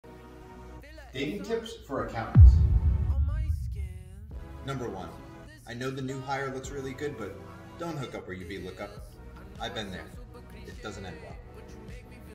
DatingTips for Accountants - 1
Looking for love after this busy season? FloQast Studios has you covered! Our very own, @therappingcpa tells you how to excel at dating with this new dating tip series!

Have your own punny piece of business dating advice? Drop it in the comments below!